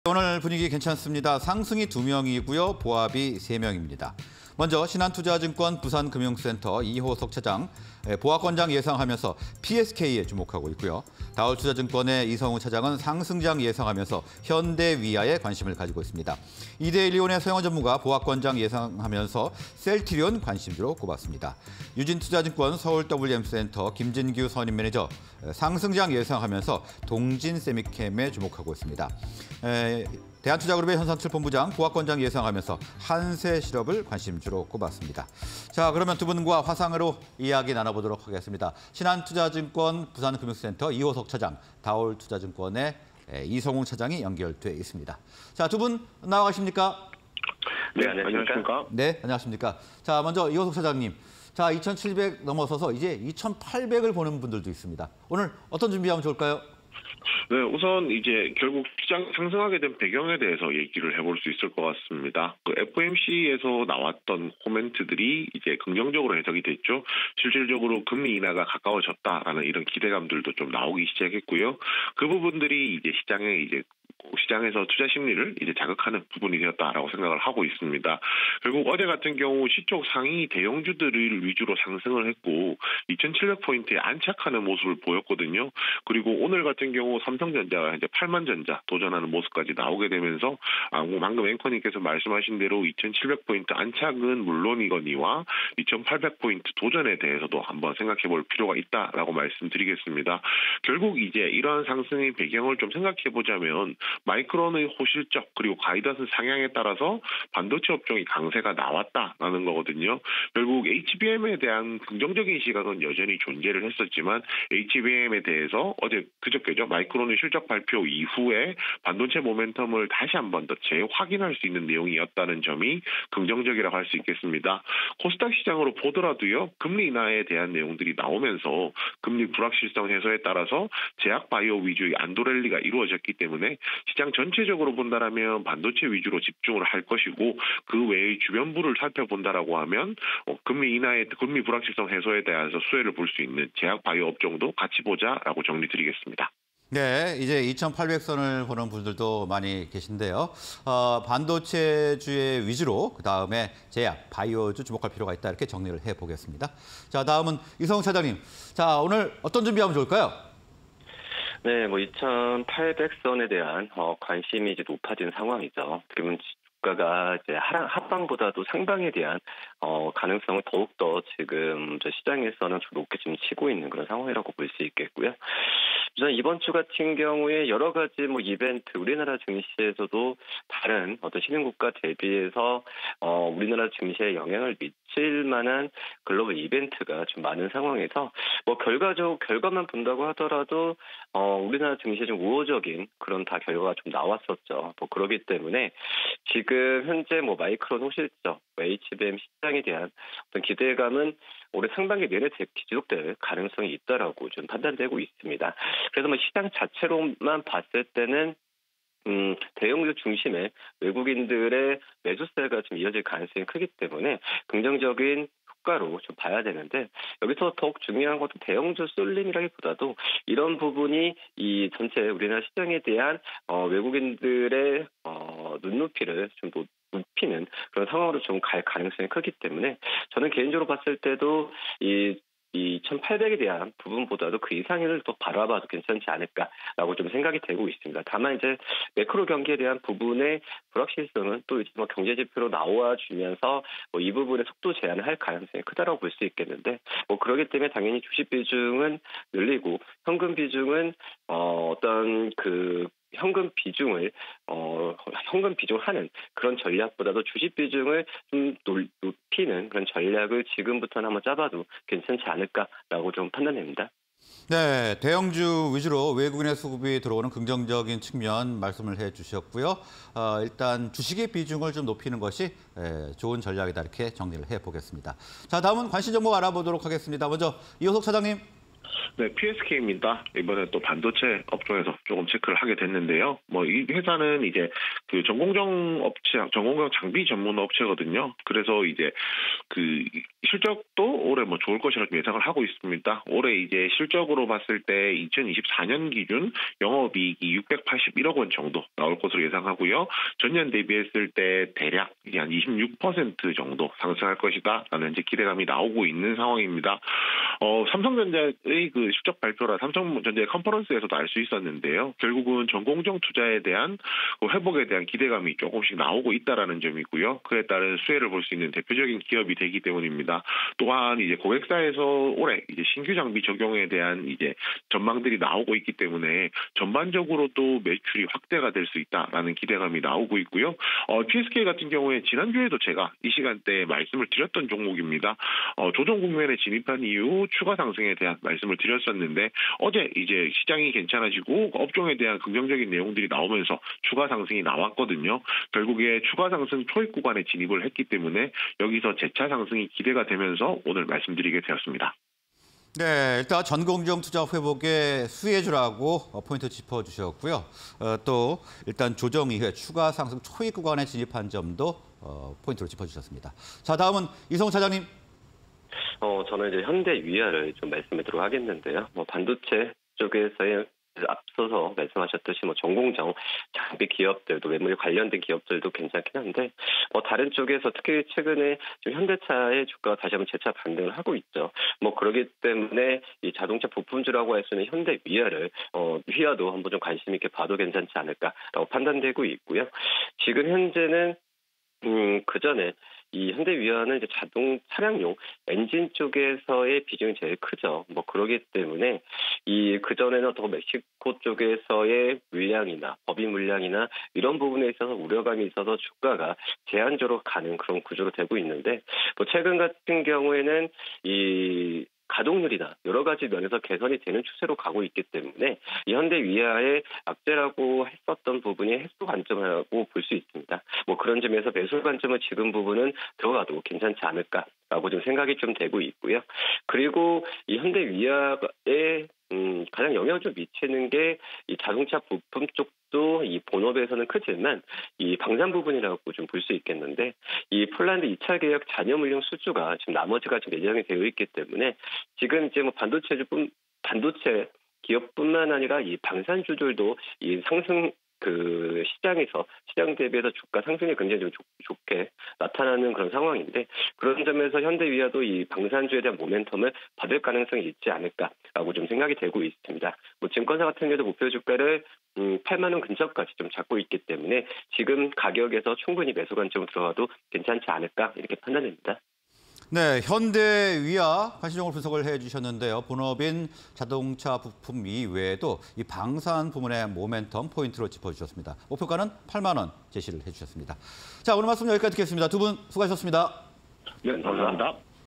피에스케이(319660)... 올해 영업익 681억원 전망... 전년비 +26% / 현대위아... 1분기 영업익 630억원대 예상_UP&DOWN 5인의 투자 전략 (20240322
Q. 코스피, 내친 김에 2800선 돌파할까?
Q. 돌아온 외국인... 코스피 힘 받나?

피에스케이(319660)
- 반도체 전공정 장비 전문
- 올해 영업익 681억원 전망... 전년비 +26%
- 전공정 투자 회복 수혜 기대
- 고객사 신규장비 적용 전망
- 조정 이후 추가상승 초입 구간

# 현대위아(011210)
- 1분기 영업익 630억원대 예상
- 산동·러시아 법인 매출 증가 예상
- 방산 부문 폴란드 잔여 물량 수주